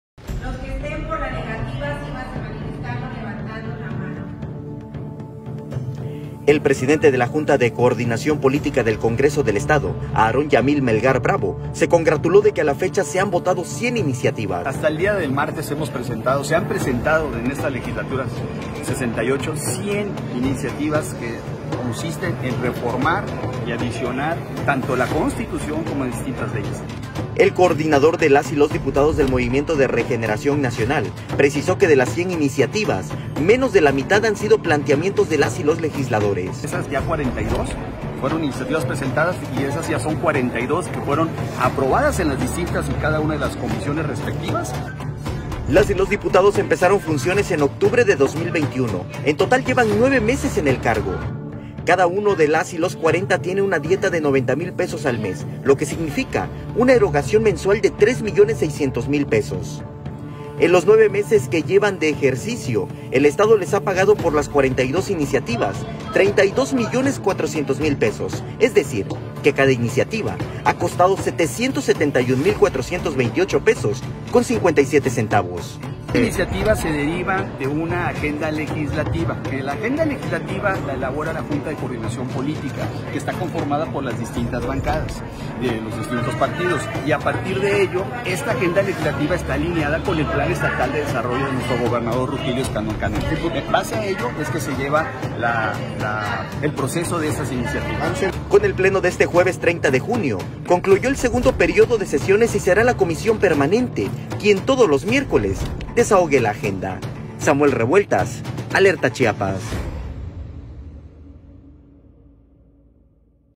Chiapas ha pagado a diputados 32 millones por 42 iniciativas.
El presidente de la Junta de Coordinación Política del Congreso del Estado, Aarón Yamil Melgar Bravo, se congratuló de que a la fecha se han votado 100 iniciativas.